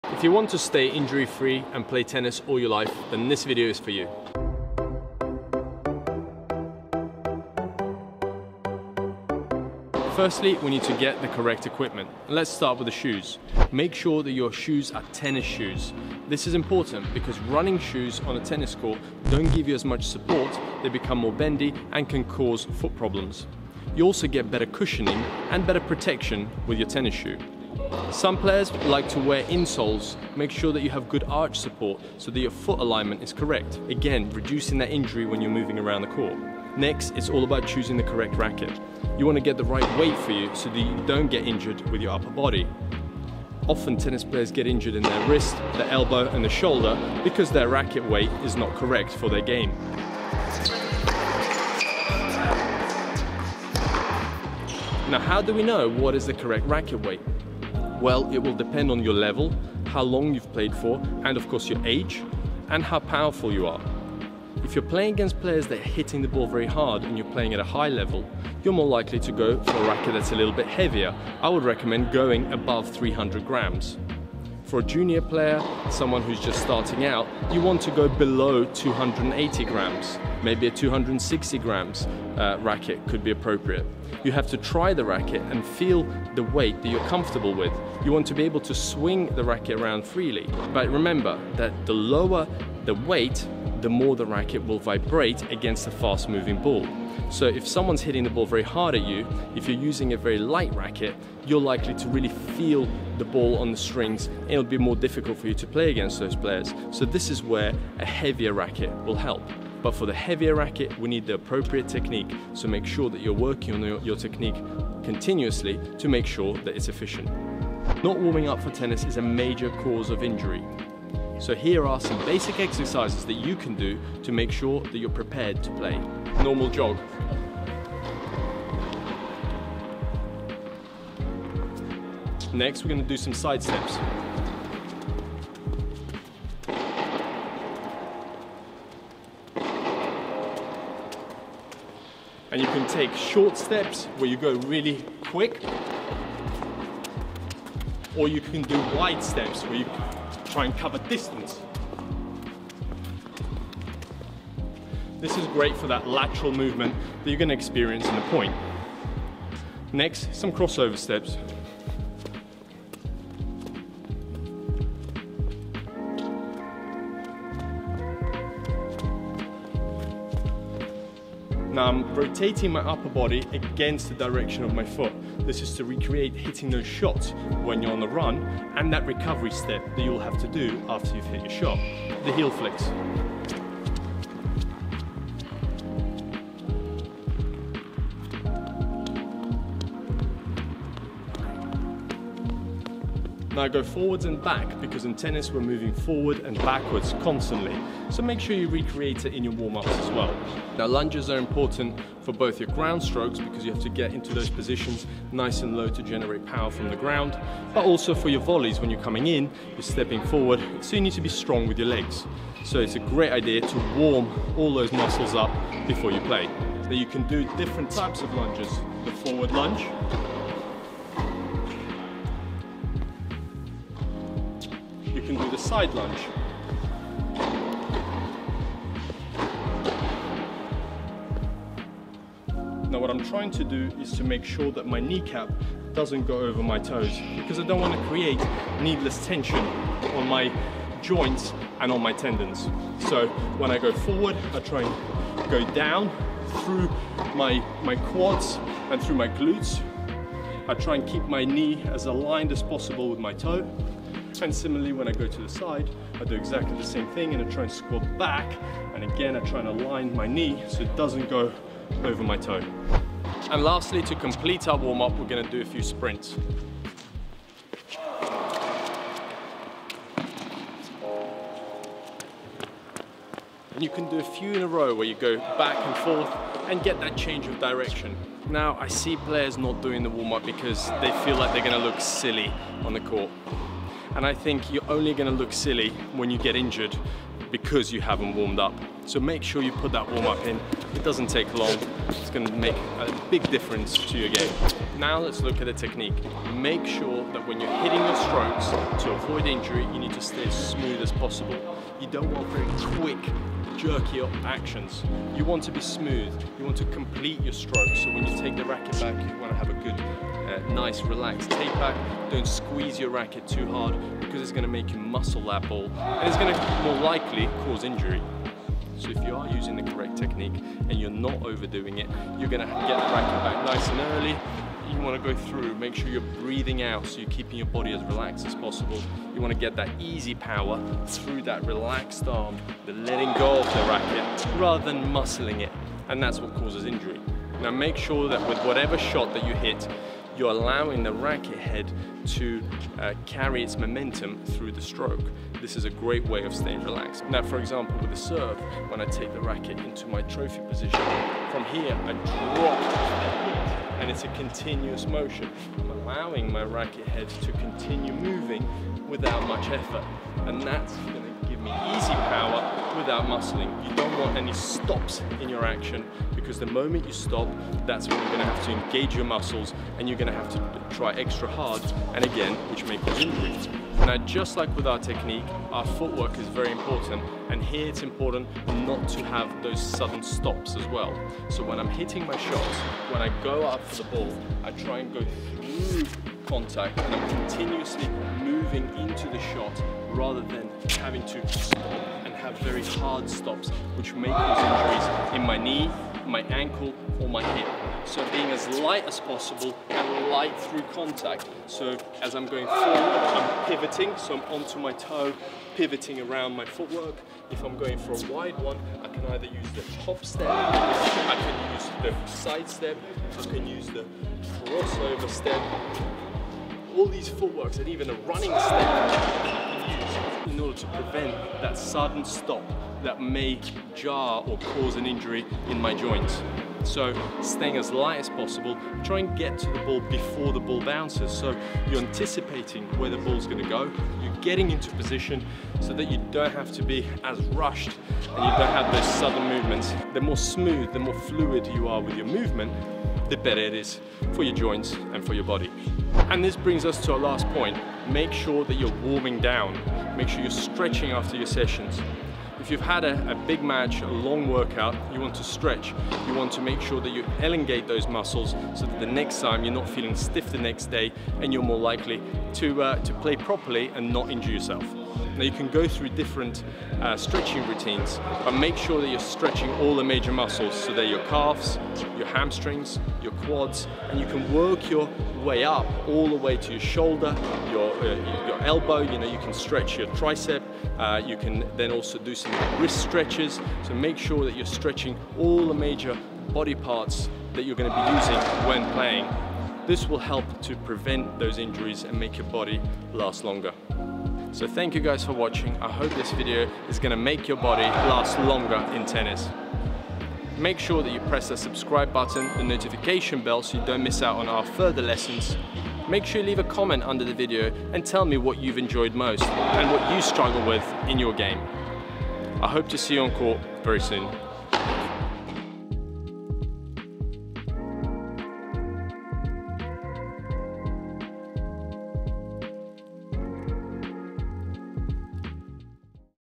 How to Stay Injury Free in Tennis
How to Stay Injury Free Playing Tennis is vital for any serious tennis player, they cause huge setbacks in both your confidence and playing level, in this video we reveal the best ways to save your body to avoid a tennis injury and play tennis for longer! Learn the correct tennis technique - Join the Forehand Revolution

In this video you will learn about the correct tennis equipment and weight of tennis racket to stop getting injured, the correct tennis warm up routine you can do before every session and the tennis footwork patterns you should do and which ones to avoid to stay fit for tennis (tennis fit).

00:00 Injury Free Tennis
00:12 Tennis Equipment
02:02 Exercises for tennis
04:55 Perfect tennis technique
06:16 Perfect tennis footwork
07:30 Tennis warm down